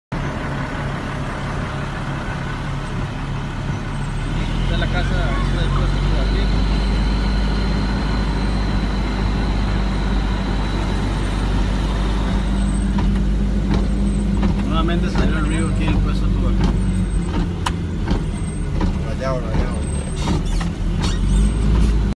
Vuelven inundaciones al Caribe sur de Costa Rica
Video del Cuerpo de Bomberos donde se pueden ver la nuevas inundaciones por aguaceros en el Caribe sur de Costa Rica.